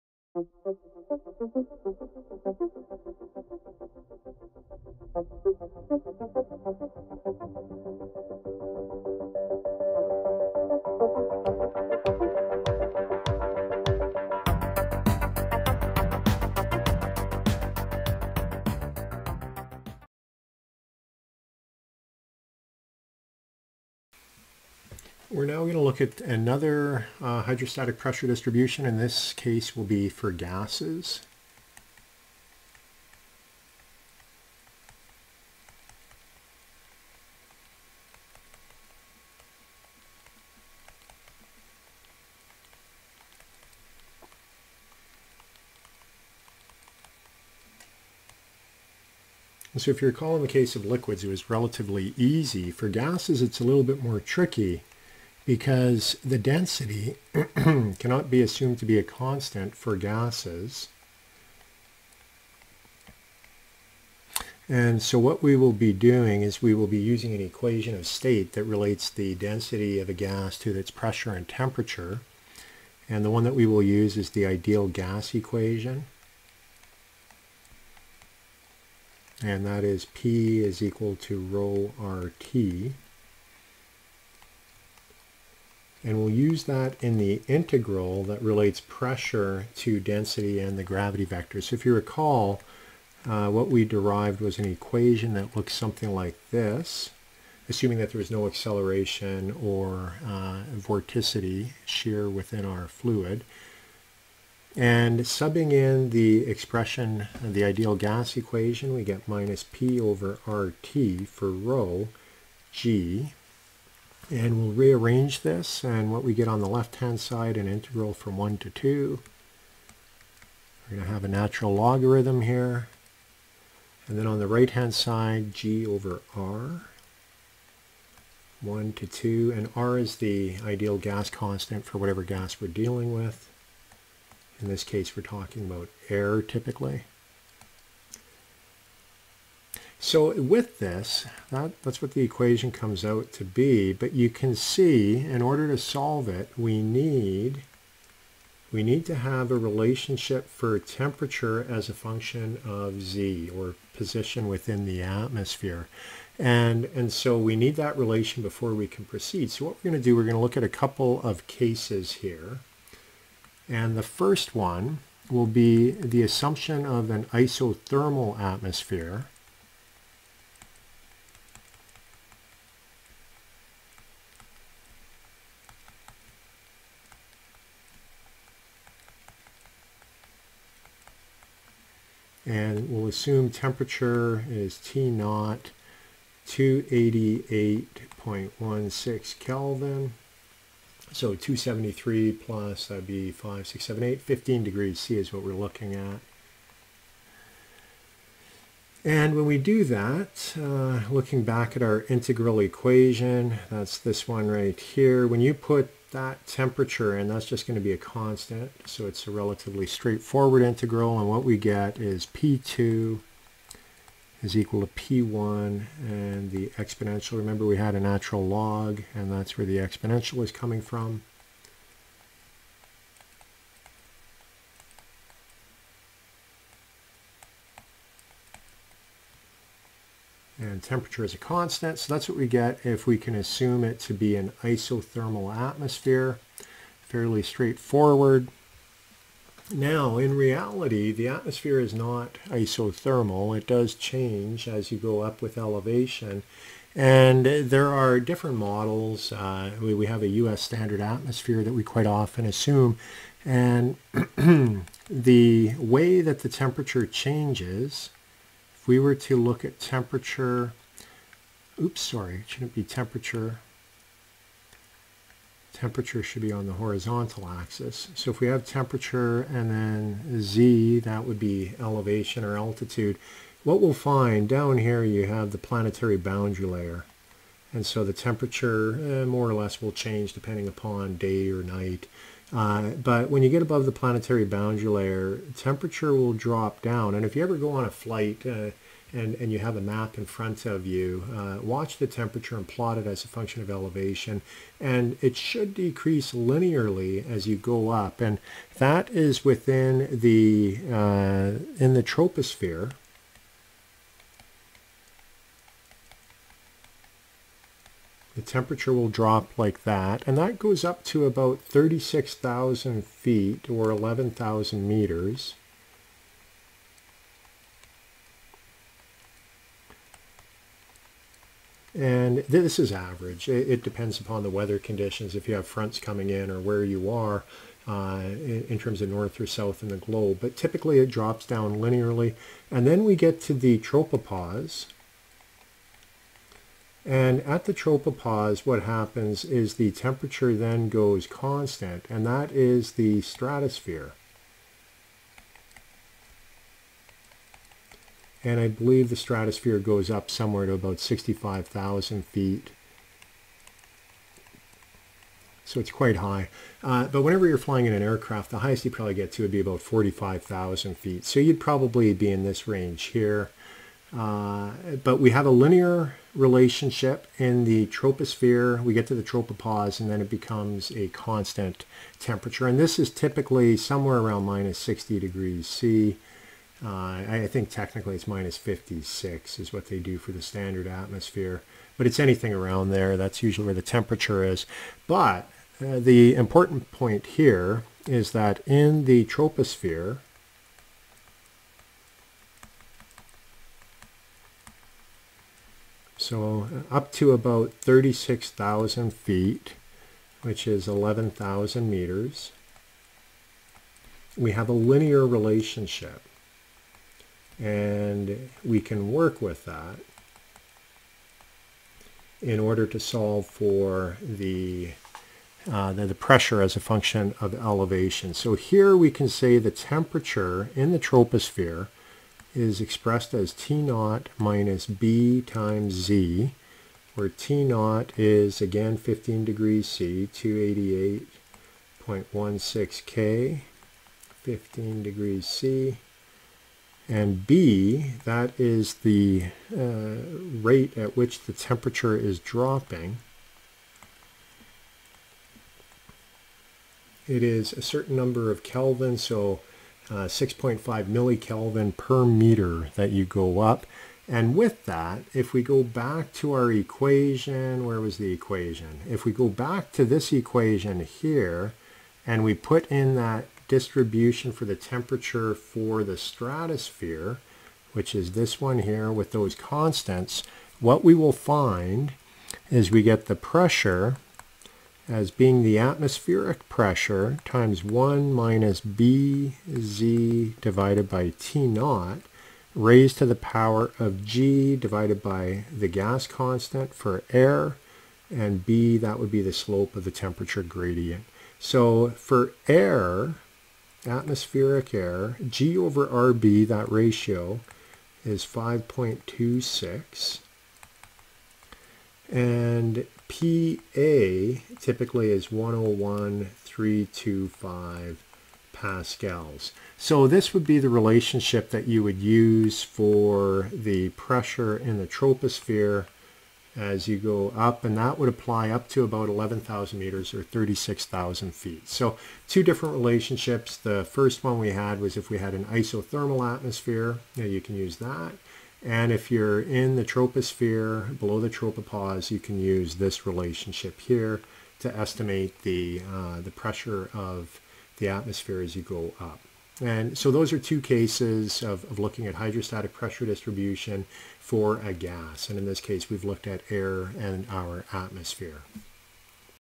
Introductory Fluid Mechanics L4 p8 Hydrostatic Pressure Distribution Gases Lecture
Fluid Statics